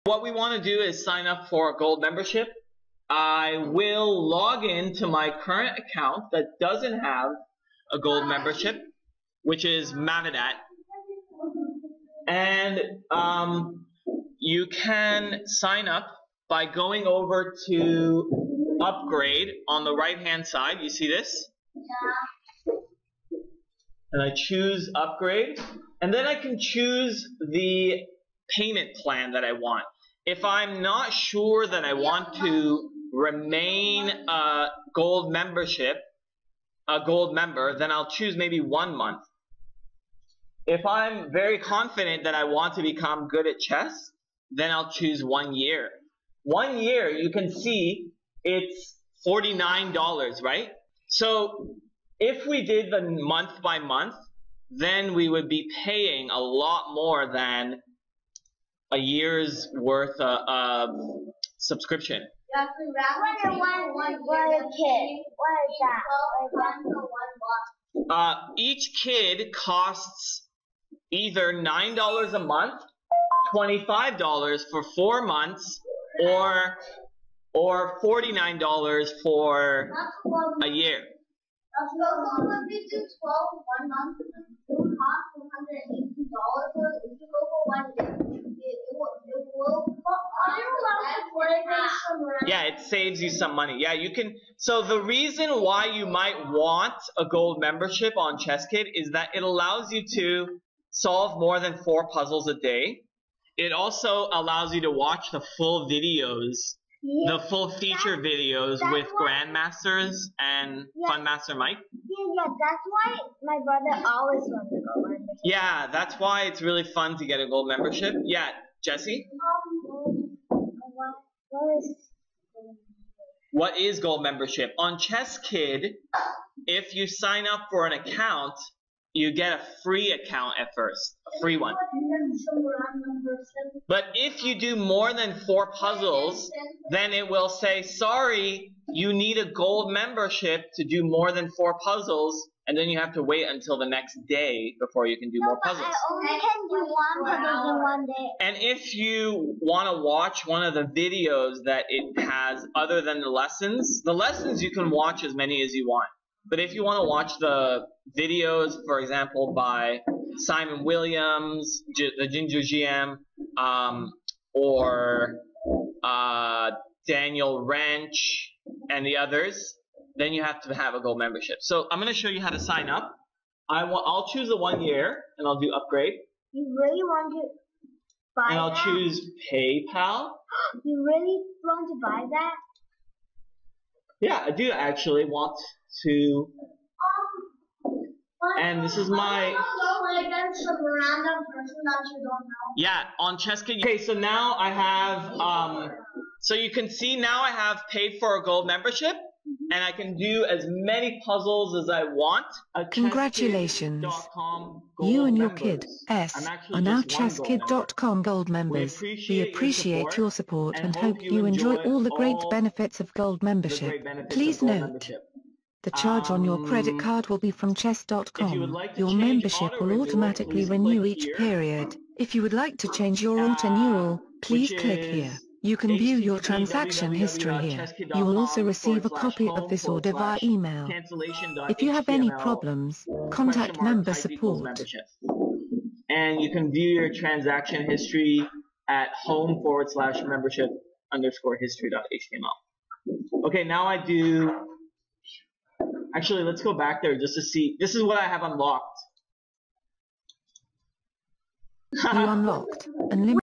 Chesskid: How to Upgrade to Gold Membership
In this video, I show the step-by-step procedure for upgrading to gold membership on chesskid.com